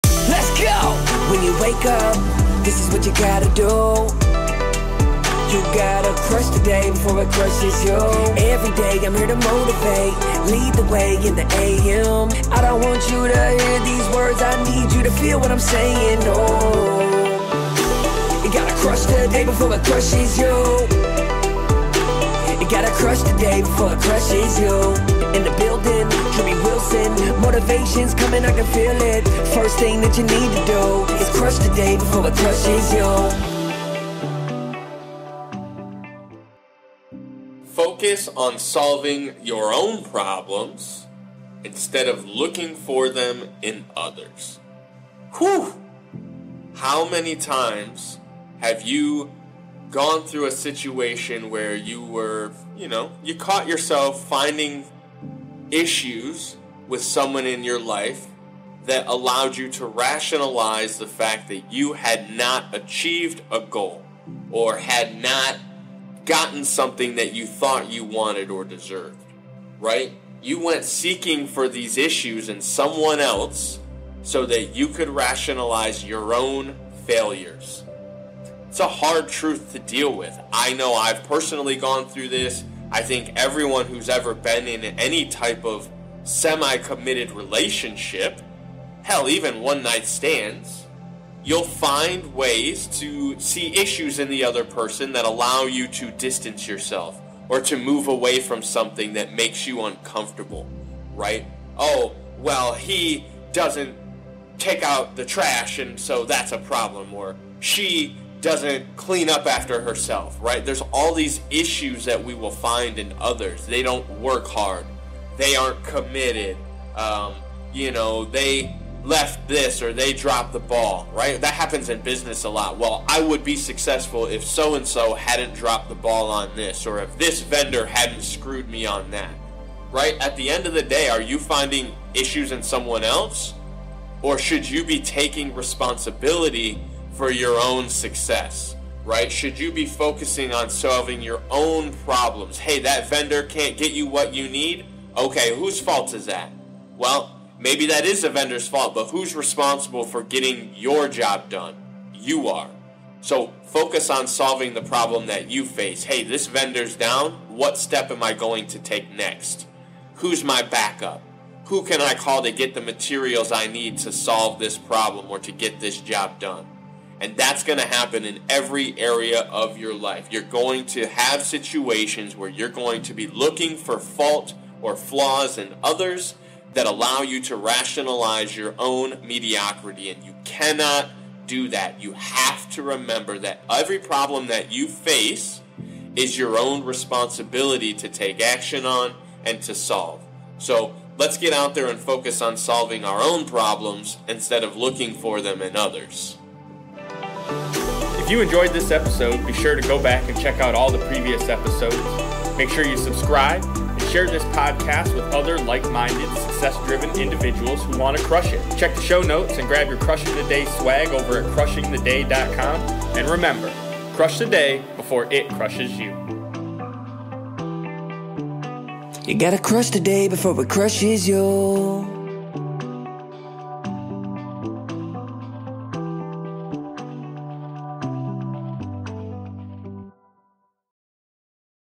Seeing The Problems | CTDP 268
Has anyone ever dropped the ball on a business agreement, making your life harder? Have you ever felt like the victim of being with a partner that fell short of your living standards? Did you blame your failures on their "shortcomings"?

On this episode of “Crushing The Day,” we are reminded that sometimes we seek out issues in others in order to rationalize the fact that we have yet to achieve our own goals. It doesn’t matter what others do, success is ultimately up to us.

Every problem we face is our responsibility to find the solution, no matter how much we want to find fault in those around us. We must become better problem solvers in all areas of our lives. Tune in to learn more.

What You’ll Learn:

  •  How it’s hard to face our own mediocrity.

  •  Why it’s easy to block dealing with our issues and place blame on others.

  •  That every problem we face is our responsibility to find a solution for.

Favorite Quote:

“ Let’s get out there and get focused on solving our own problems Instead of looking for them in others!”

 —Drewbie Wilson